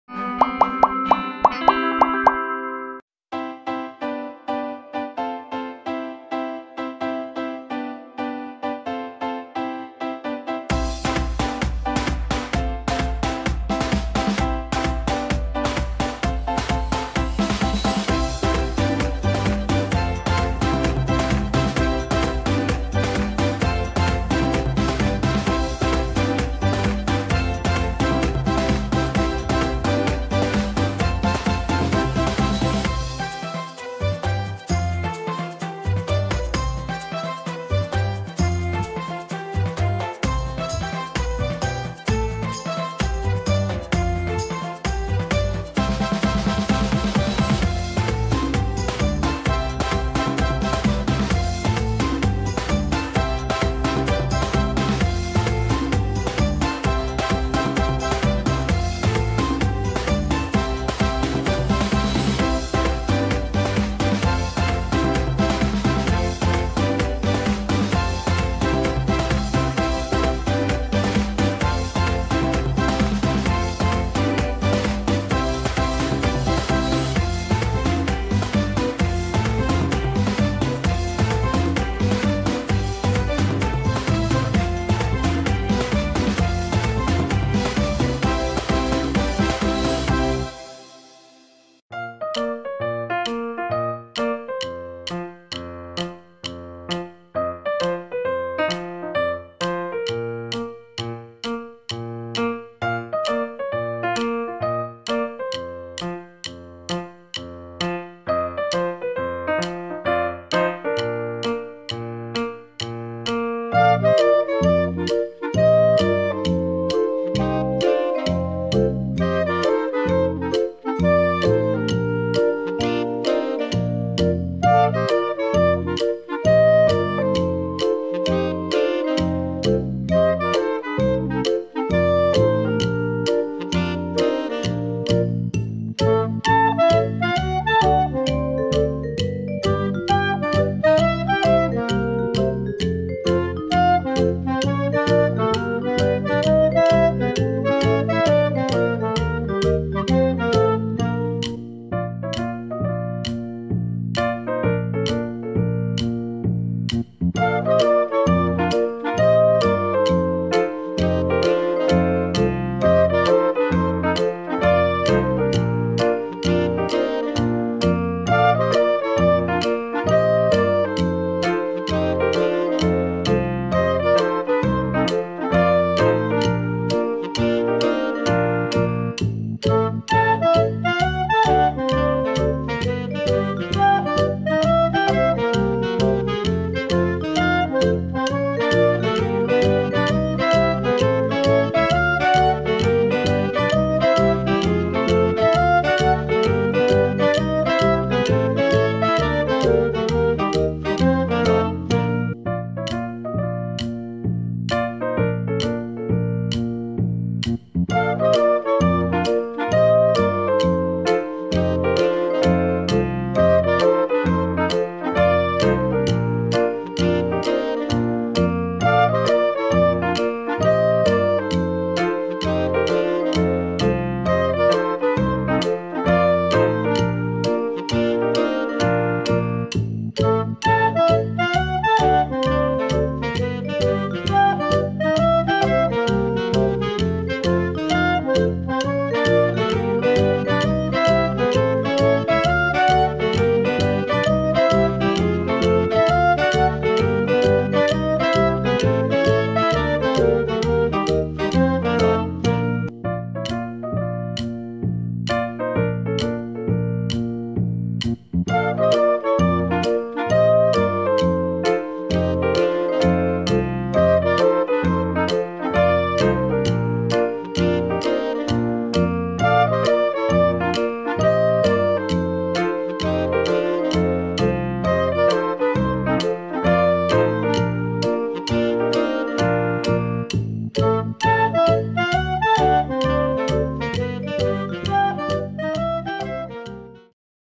Пчела из пластилина Play Doh - Обучающее видео для детей mirglory
Как слепить пчелу из пластилина? Смотрите новое обучающее видео для детей где показано что получится из Play Doh.
 Лепим из пластилина пчелу.

Новые развивающие мультики для маленьких детей про машинки, транспорт,
 животных смотрите на нашем канале mirglory